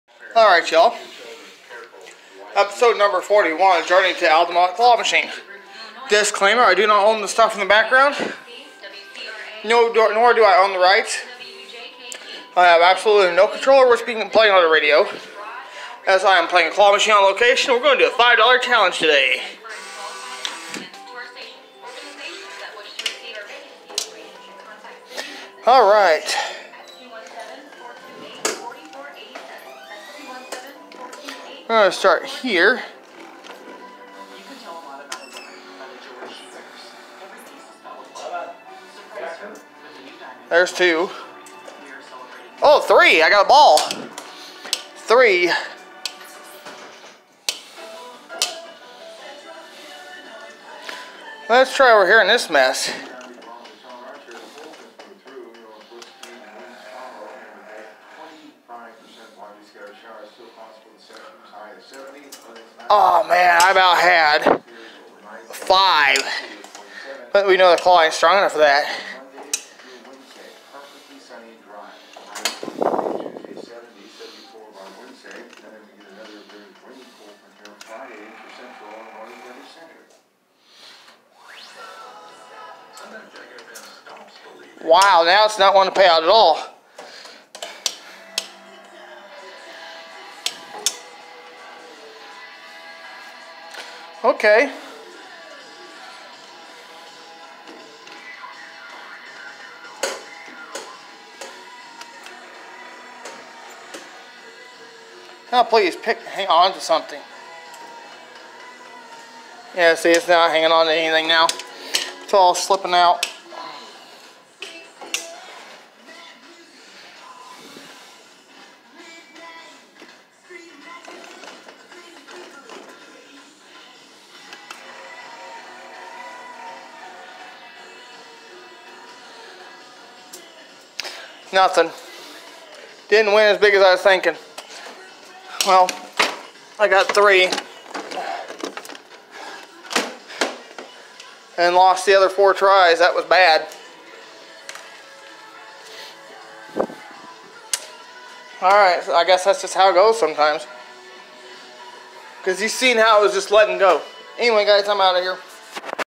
Journey To The Altamont Claw Machine Episode 41 5 dollar challenge 3in1
Today i head back to Joe's Pizza in Altamont Illinois for episode 41 of journey to the altamont claw machine now first try was easy after that it started rigging me out of my wins which means claw isn't as strong as i was orginally thinkin